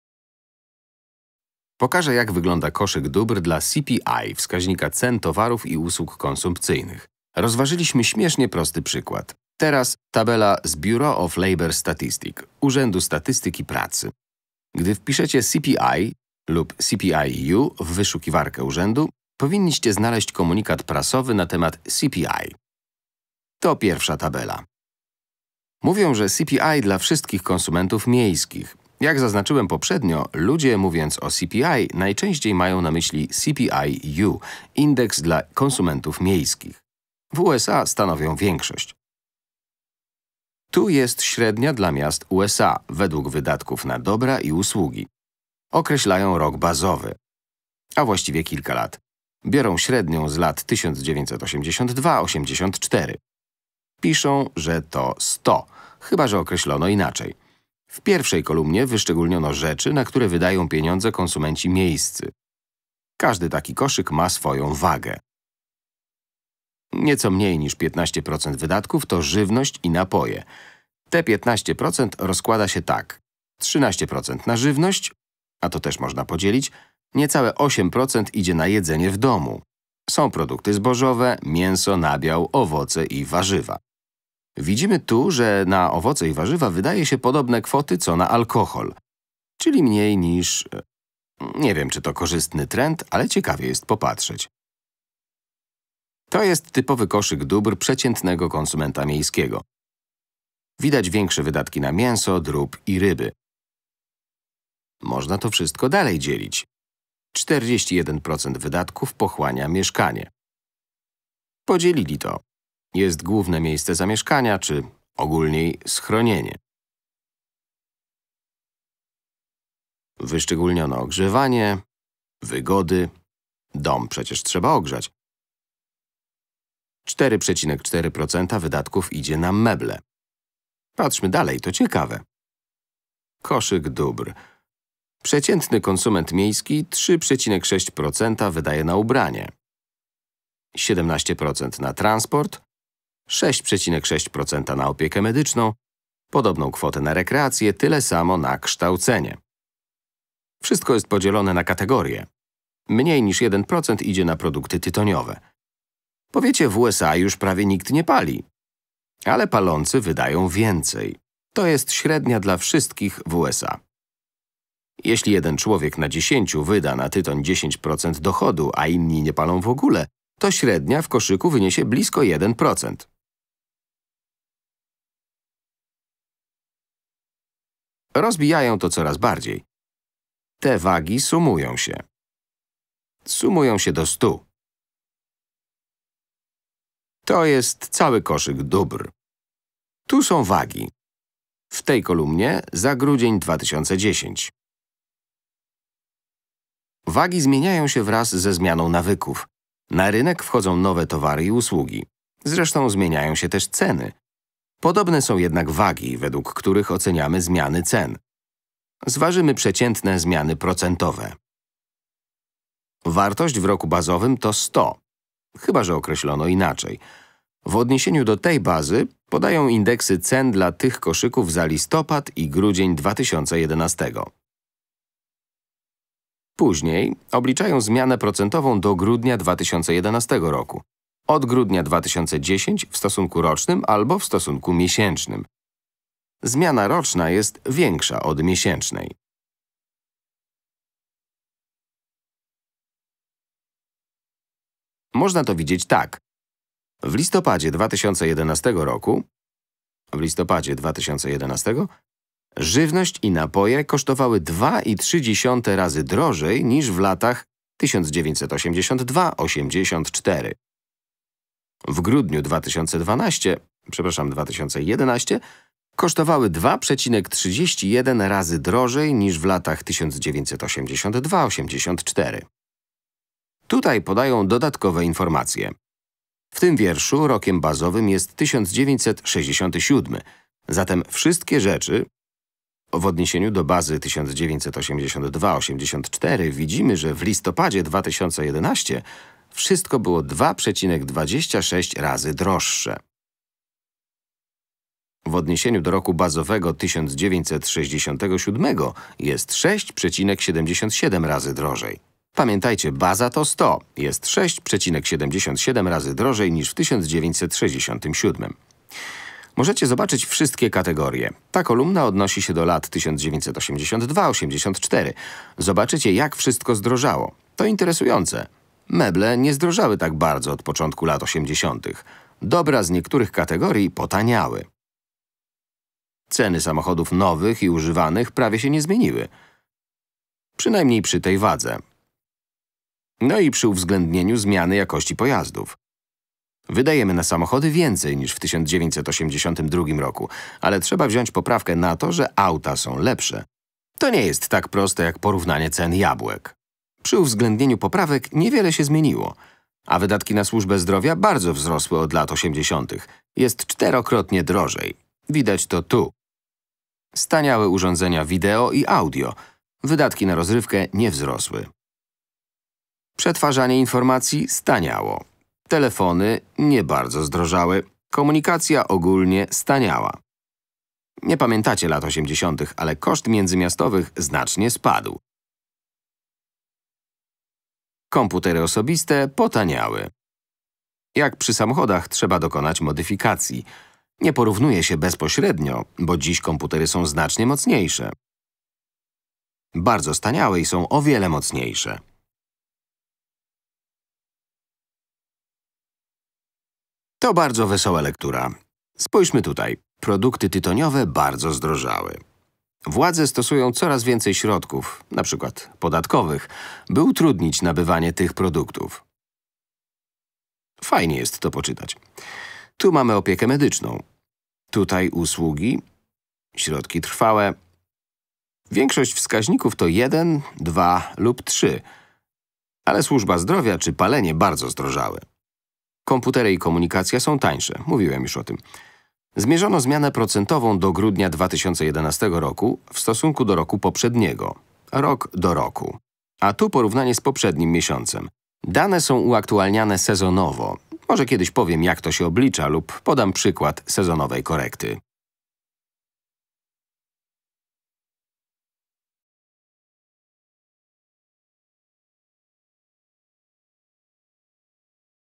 Makroekonomia 12/66. Indeks cen konsumeckich - koszyk dóbr konsumenta miejskiego
Konsultacja merytoryczna: Marcin Senderski (Akademia Mediów), przy  wsparciu Fundacji 2065 im. Lesława A. Pagi.